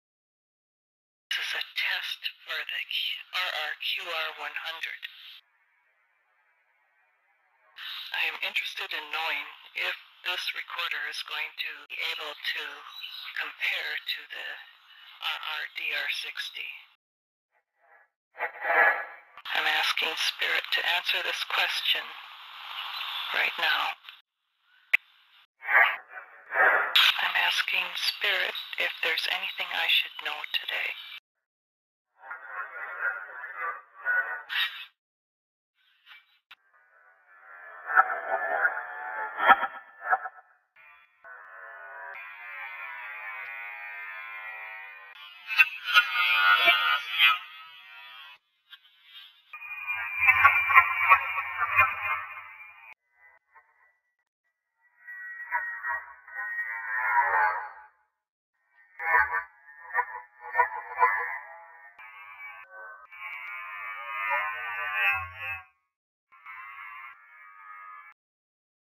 Experimenting with the RR-QR 100
I decided I would experiment with the RR-QR100 & 240. This is the 100 with audio enhancement